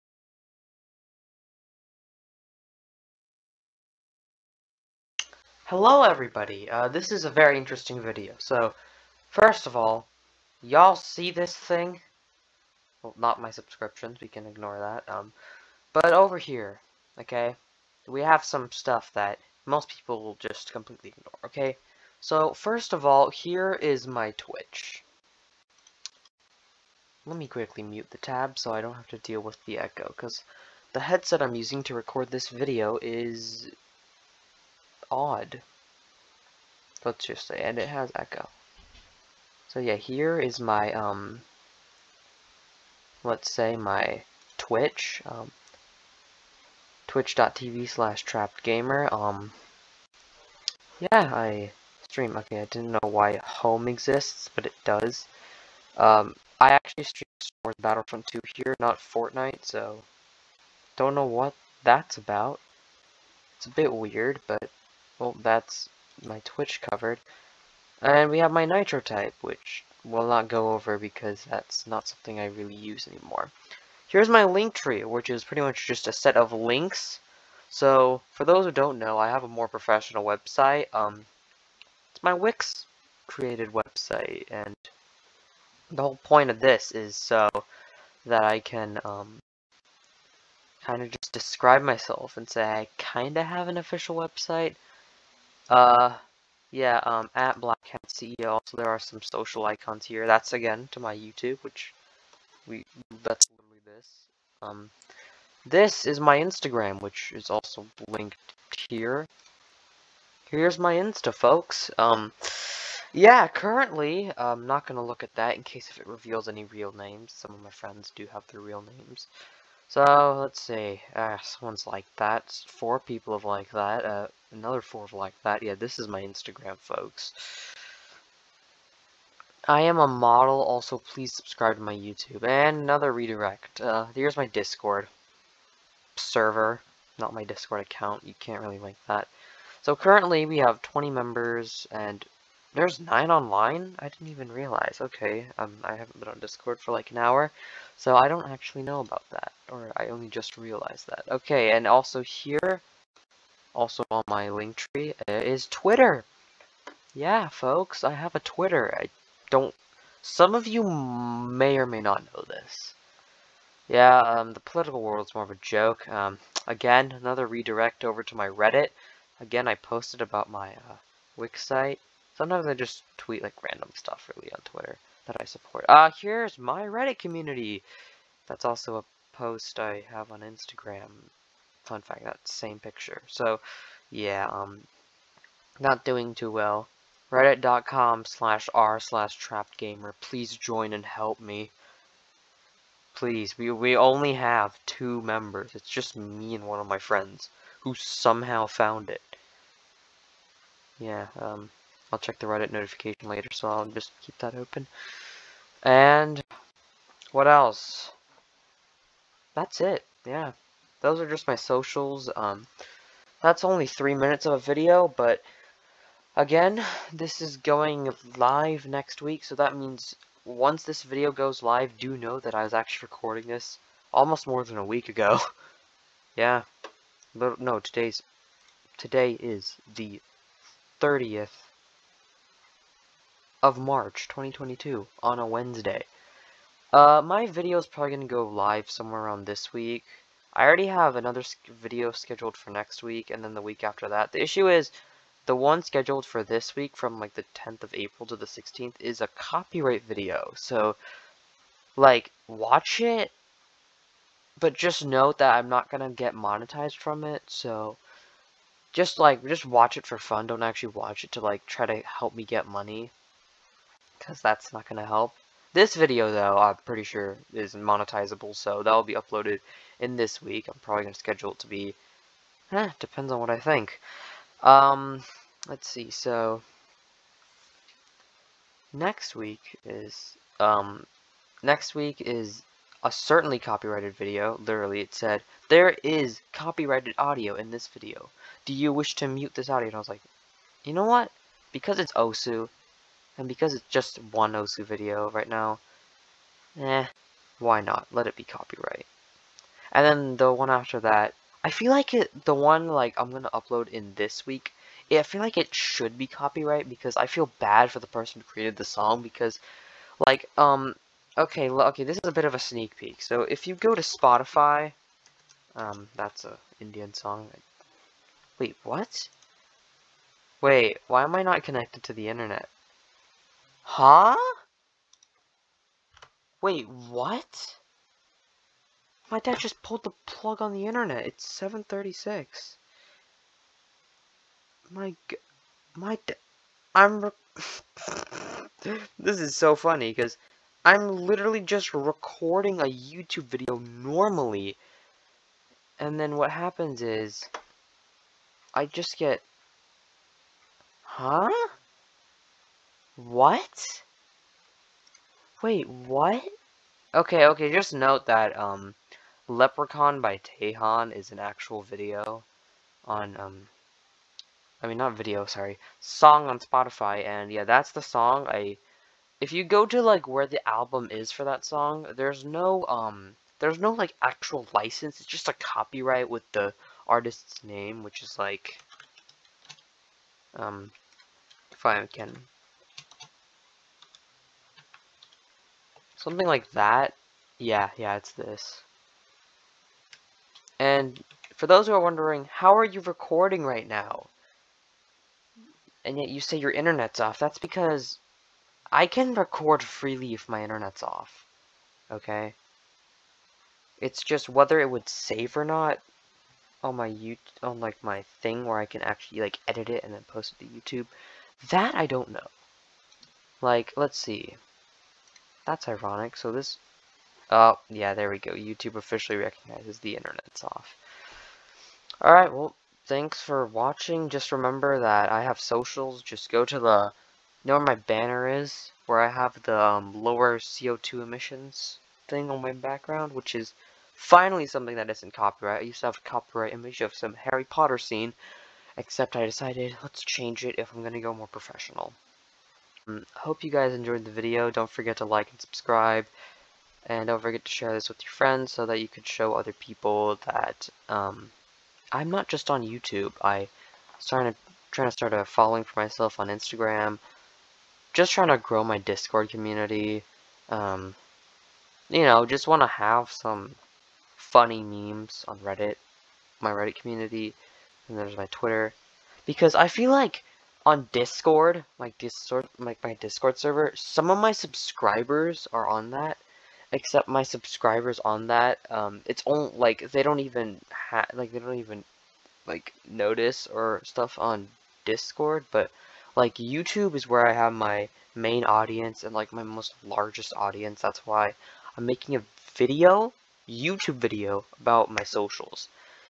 Talking about my socials outside of YouTube
This video was recorded on my PC. Which would be like my Twitter, Instagram, Osu! profile (that's not a social), Twitch, & more!